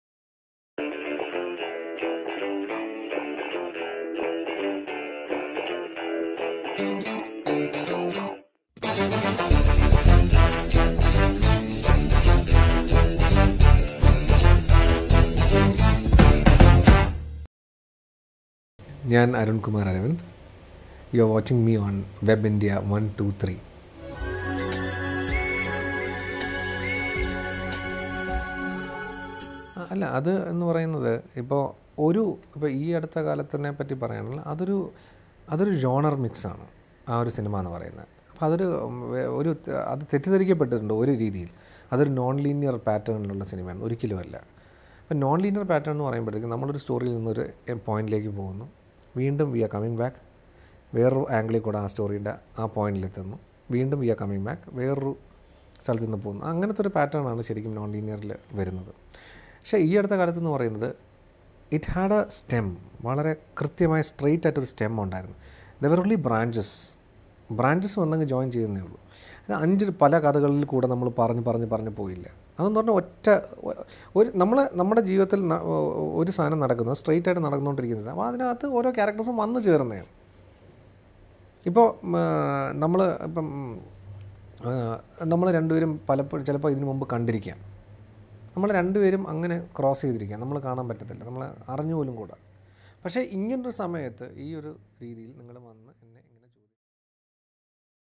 Interview with Arun Kumar Aravind
Arun Kumar Aravind  is the director of Cocktail and Ee Adutha Kaalathuand, owns to his credit a span of 11 years experience as a Film Editor in this industry.Arun Kumar has been director Priyadarshan's, principal editor from 'Vettam' (2004) onwards before he turned to filmmaking with Cocktail. He edited Kanchivaram which got National Award for the Best Film in 2008. He had also worked with other well- known directors like T.K Rajeev Kumar, Shaji Kailas, Suresh Krishna.